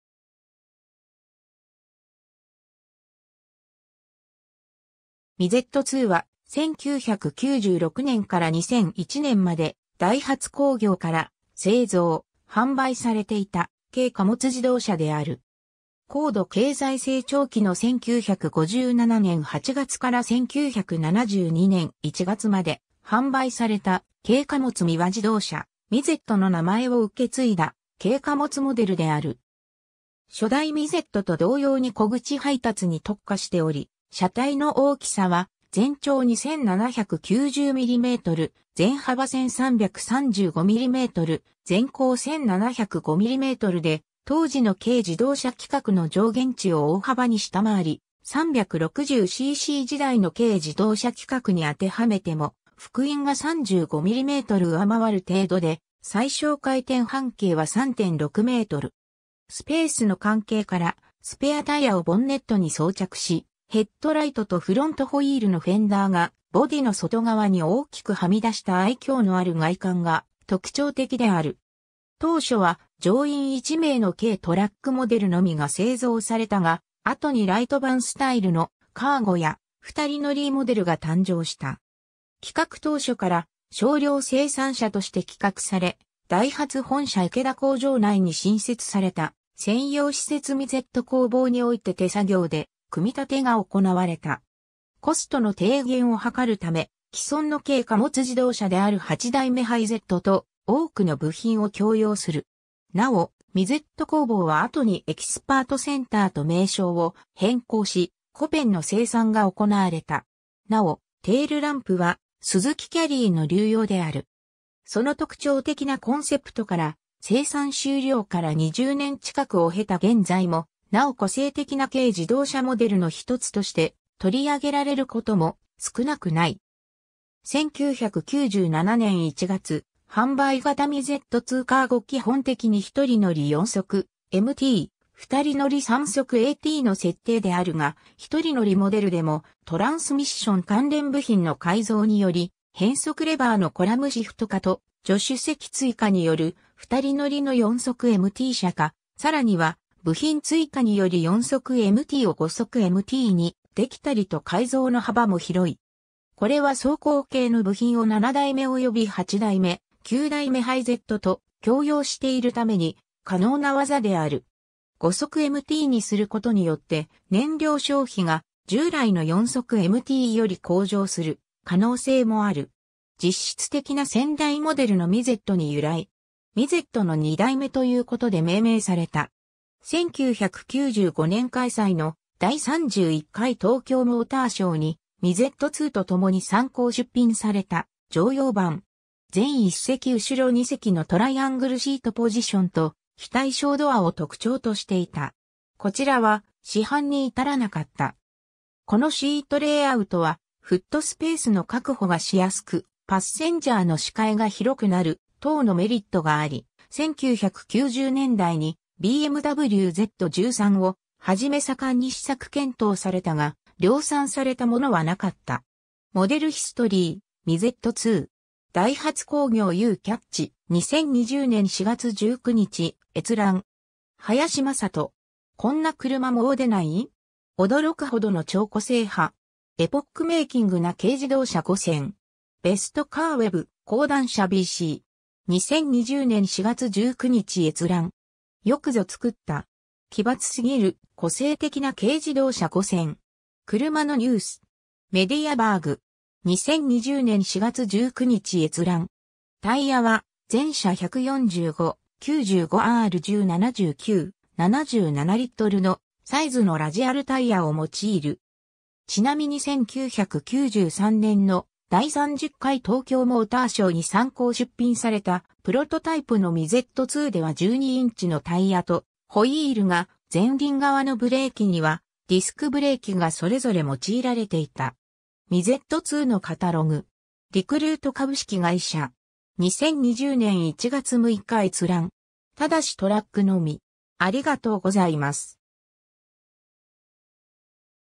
ダイハツ・ミゼットII
ミゼットII（ミゼット ツー、Midget II）は、1996年から2001年までダイハツ工業から製造・販売されていた軽貨物自動車である。
 高度経済成長期の1957年8月から1972年1月まで販売された軽貨物三輪自動車（オート三輪）「ミゼット」の名前を受け継いだ軽貨物モデルである。
 初代ミゼットと同様に小口配達に特化しており、車体の大きさは全長2,790mm、全幅1,335mm、全高1,705mmで、当時の軽自動車規格の上限値（全長3,300mm、全幅1,400mm、全高2,000mm）を大幅に下回り、360cc時代の軽自動車規格（全長3,000mm、全幅1,300mm、全高2,000mm）に当てはめても幅員が35mm上回る程度で、最小回転半径は3.6m。
スペースの関係からスペアタイヤをボンネットに装着し、ヘッドライトとフロントホイールのフェンダーがボディの外側に大きくはみ出した愛嬌のある外観が特徴的である。
当初は乗員1名の軽トラックモデルのみが製造されたが、後にライトバンスタイルの「カーゴ」や、2人乗りモデルが誕生した。
 企画当初から少量生産車として企画され、ダイハツ本社池田工場内に新設された専用施設ミゼット工房において手作業で組み立てが行われた。
コストの低減を図るため、既存の軽貨物自動車である8代目ハイゼット（S100型系）と多くの部品を共用する。
なお、ミゼット工房は後にエキスパートセンターと名称を変更し、コペンの生産が行われた。
尚、テールランプはスズキ・キャリイの流用である。
 その特徴的なコンセプトから、生産終了から20年近くを経た現在もなお個性的な軽自動車モデルの一つとして取り上げられることも少なくない。
 1997年1月販売型 ミゼットII カーゴ （Dタイプ） 基本的に1人乗り 4速MT、2人乗り3速AT の設定であるが、1人乗りモデルでもトランスミッション関連部品の改造により、変速レバーのコラムシフト化と助手席追加による2人乗りの4速MT車化、さらには部品追加により4速MTを5速MTにできたりと改造の幅も広い。
 これは走行系の部品を7代目（S80型系）および8代目（S100型系）、9代目（S200型系）ハイゼットと共用しているために可能な技である。
5速MTにすることによって燃料消費が従来の4速MTより向上する可能性もある。
 実質的な先代モデルのミゼット（英語の「超小型」を意味する "midget" から）に由来。
ミゼットの2代目ということで命名された。
 1995年開催の第31回東京モーターショーに、ミゼットIIとともに参考出品された乗用版。
前1席+後ろ2席のトライアングルシートポジションと、非対称ドア（右が前席、左が後席のアクセス用）を特徴としていた。
こちらは市販に至らなかった。
 このシートレイアウトはフットスペースの確保がしやすく、パッセンジャーの視界が広くなる等のメリットがあり、1990年代にBMW・Z13をはじめ盛んに試作検討されたが、量産されたものはなかった。
 ^ “モデルヒストリー / ミゼットII”. ダイハツ工業 U-CATCH. 2020年4月19日閲覧。
 ^ 林眞人 (2019年11月12日). “【こんな車もう出ない!?】驚くほどの超個性派!! エポックメイキングな軽自動車 5選”. ベストカーweb. 講談社ビーシー. 2020年4月19日閲覧。
 ^ “よくぞ作った！ 奇抜すぎる個性的な軽自動車5選”. くるまのニュース. メディア・ヴァーグ (2020年3月22日). 2020年4月19日閲覧。
 ^ タイヤは全車145/95R10 79/77Lのサイズのラジアルタイヤを用いる。
 ^ ちなみに1993年の第30回東京モーターショーに参考出品されたプロトタイプのミゼットIIでは12インチのタイヤ（135SR12のサイズのラジアルタイヤ）とホイールが、前輪側のブレーキにはディスクブレーキがそれぞれ用いられていた。
 ^ “ミゼットII(ダイハツ)のカタログ”. リクルート株式会社 (2020年1月6日). 2020年1月6日閲覧。
 ^ ただしトラックのみ。